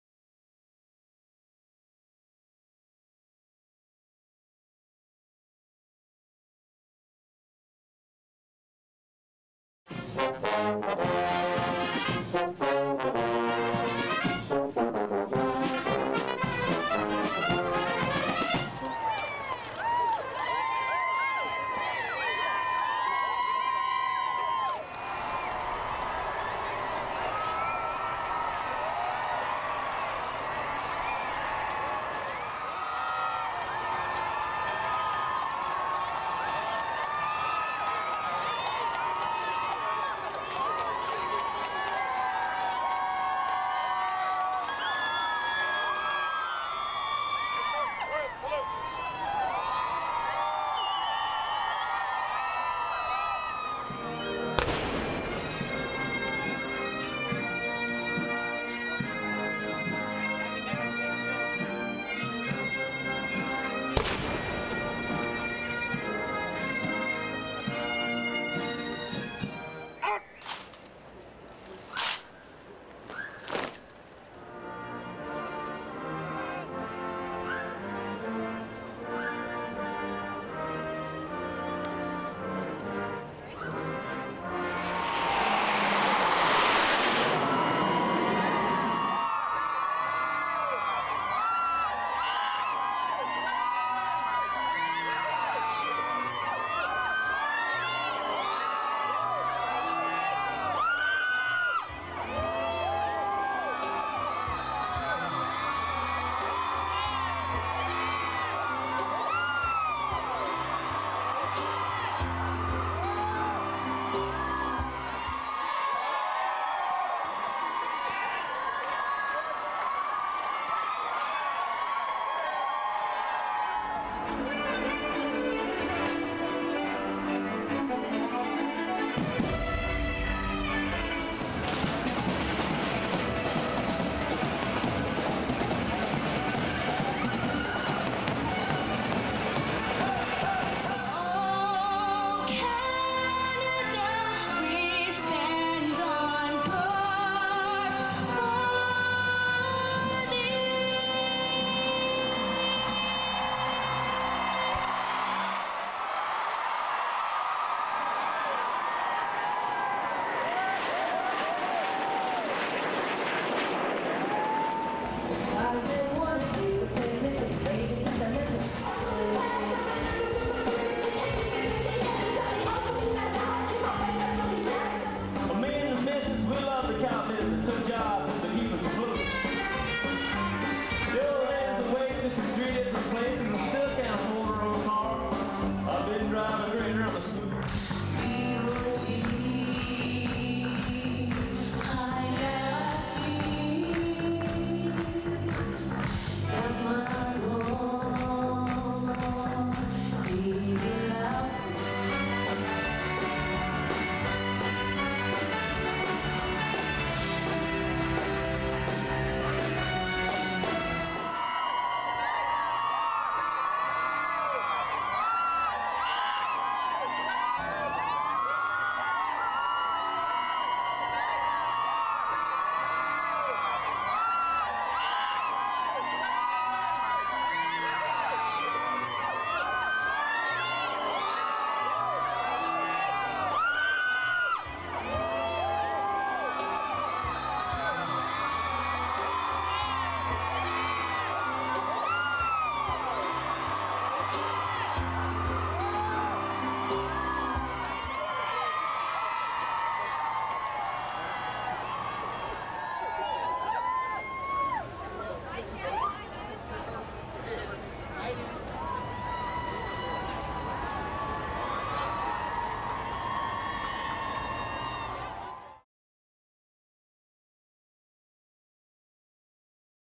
Royal Tour 2011 - Canada Day in Ottawa - The Duke and Duchess of Cambridge
Canada Day Parliament Hill Noon Show

The Government of Canada is honoured that Their Royal Highnesses The Duke and Duchess of Cambridge (Prince William and Kate Middleton) are in Canada on their first international tour as a married couple. The Royal Couple celebrated their wedding at Westminster Abbey on April 29, 2011. The 2011 Royal Tour of Canada provides Canadians with the opportunity to highlight their history, traditions, and shared values, while also strengthening our Canadian identity.